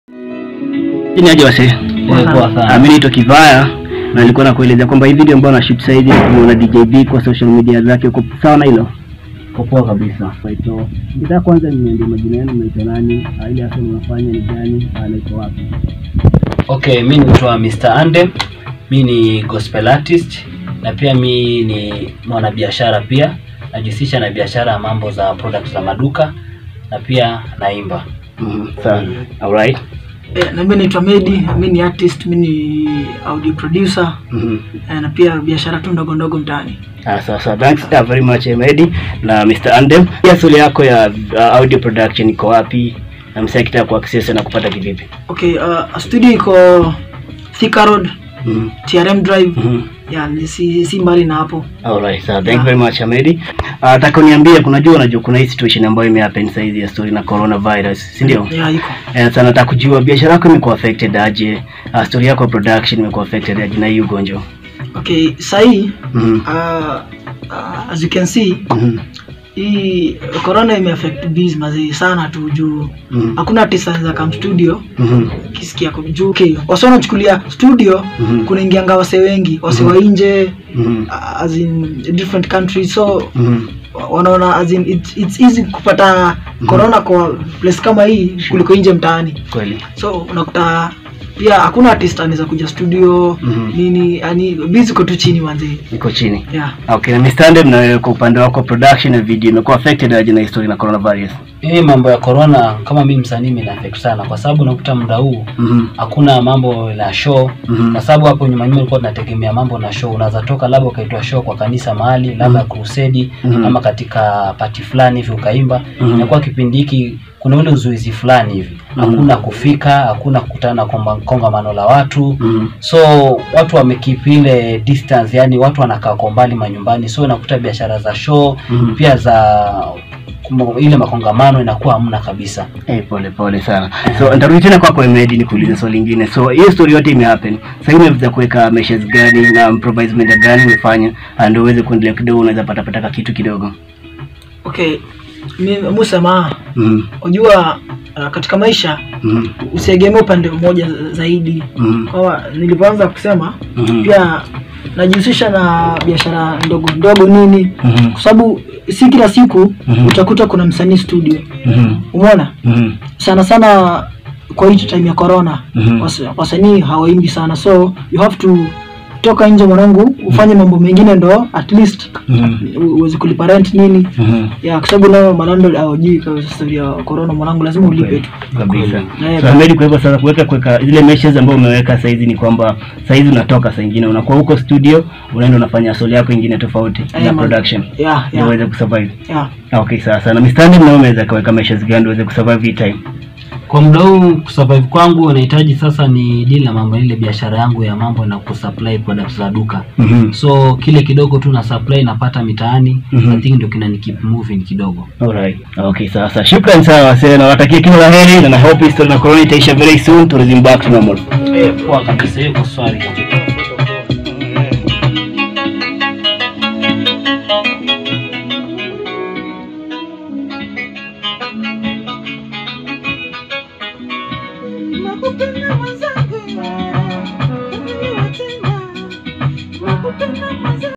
Kivaya Artist Interview | Producer Hemedy and Artist Mr Andem
Producer Hemedy and Artist Andem takes us through the challenges they are facing now during this Pandemic with measures they are applying to survive this period.